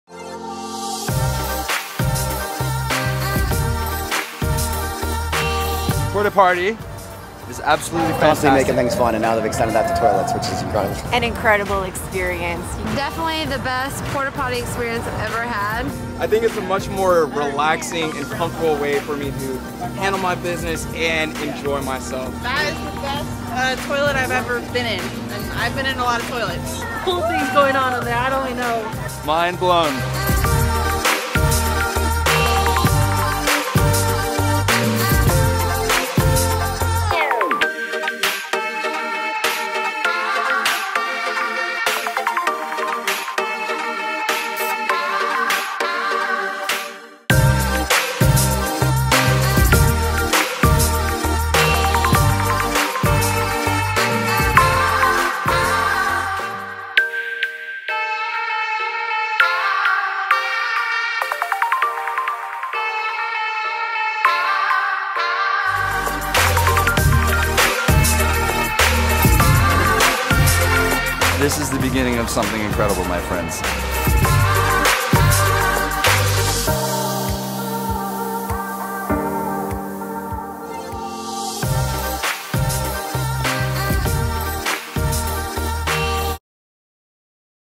Ready2Go’s PortaParty Restroom Trailer!
Elevate Your Event
A first of its kind, this state-of-the-art restroom is a party on the inside and out! Ready2go's PORTAPARTY is like no other porta-potty you've ever been in. Mindful features and interactive technology allow you to truly mix business with pleasure.

Features
• Motorized Squatty Feet
• Perfect Selfie Station
• Assorted Containers with Customizable Gumball Goodie
Treat
Soap Dispenser Voting System
 40" Waterproof Monitors with Customizable Slide Show, Videos or Music
16 ft L (Includes the tongue) × 10 ft W (Include the stairs)
 200 Gallon Fresh Water Tank
350 Gallon Waste Tank
1 Women's Toilet ,1 Urinal and 1 Men's Toilet
 LED Ceiling Lighting with wall sconces in stalls and common area